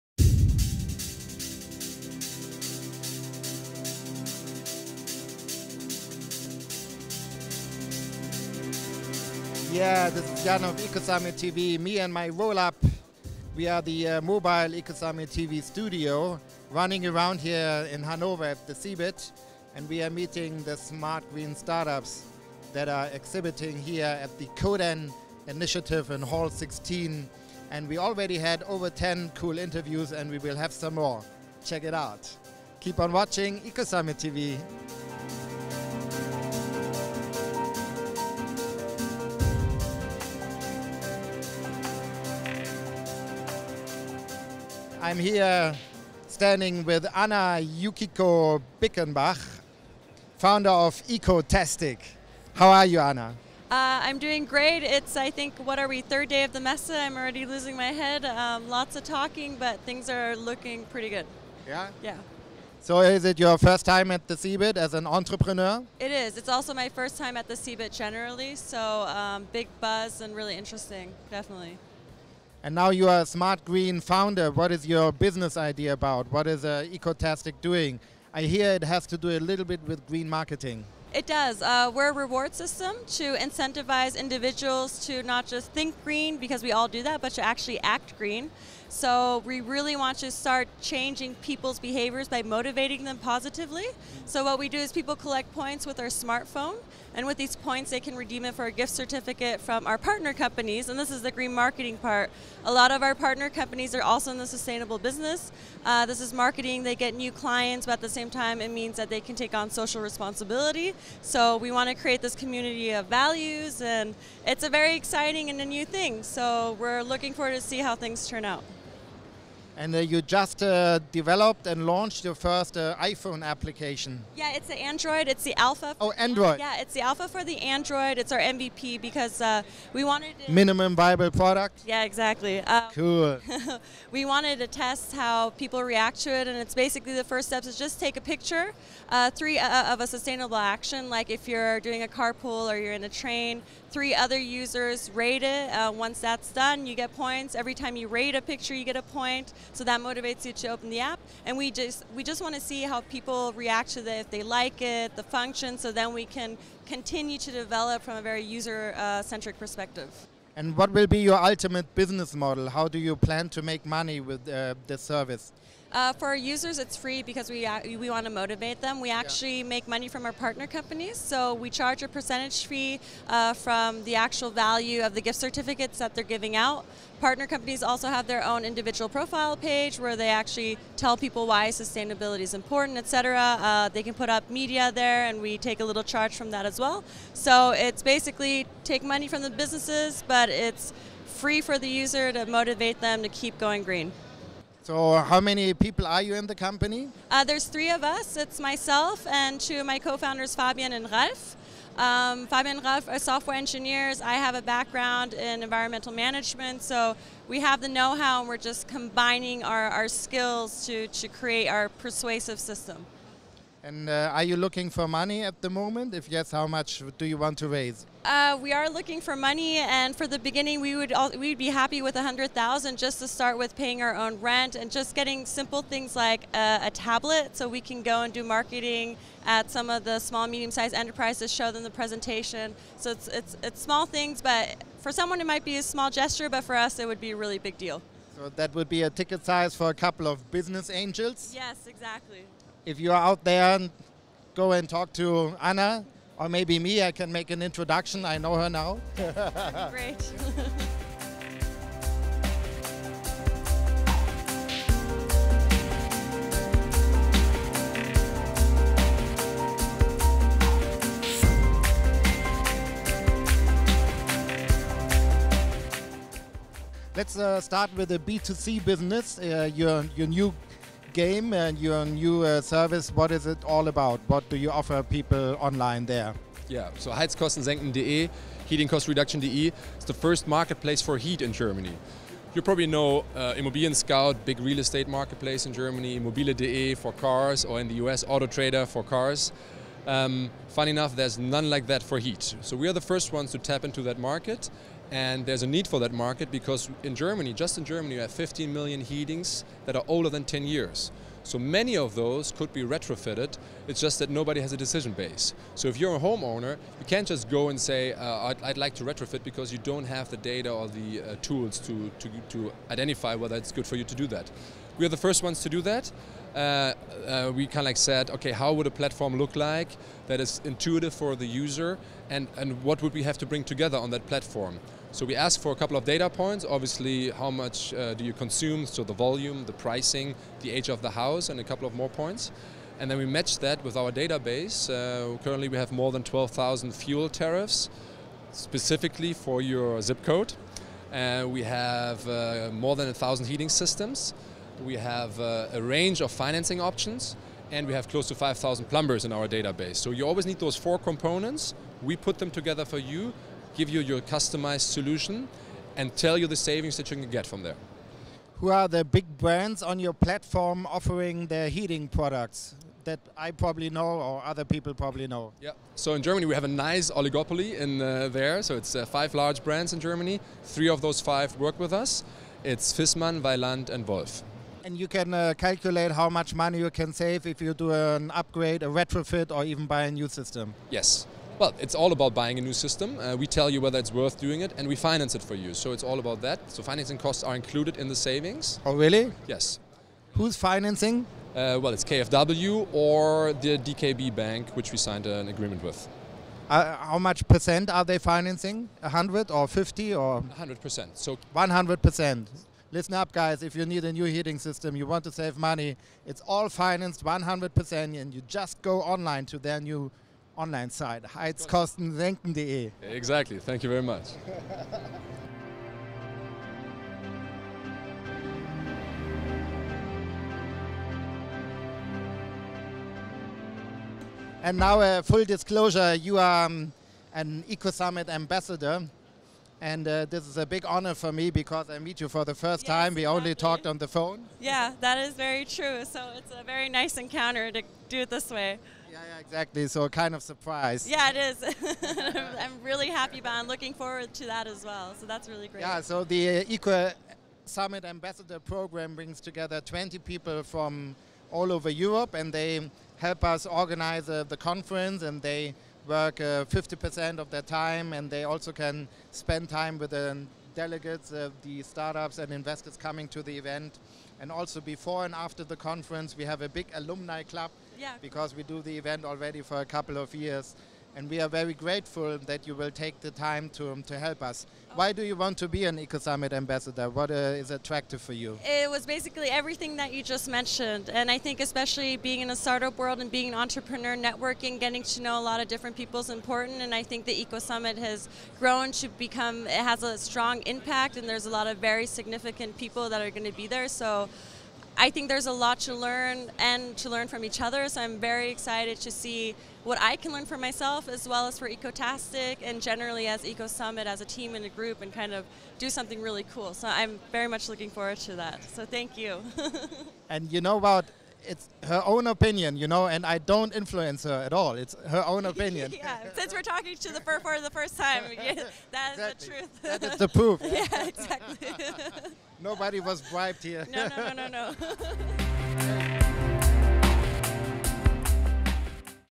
Cebit Code_n 2013: Ecotastic Futurewatt Interview
Our mobile Ecosummit TV studio stops again at CeBIT Code_n 2013 for more smart green startup interviews. This time, Jan interviews the Berlin-based founders Anna Yukiko Bickenbach of Ecotastic and Philipp Pausder of FutureWatt. Ecotastic is a new green marketing service provider that runs a reward system to foster green consumer behaviour. Consumers document their green actions with their smartphone and are rewarded with gift certificates from partner companies. FutureWatt recently launched heizkosten-senken.de (reducing heating costs), an online platform for consumers that want to buy a more efficient heating system. Around 15 million households in Germany operate old heating systems that should be replaced with a new one. At least this is the target market which Philipp and his team are dreaming about by day and by night.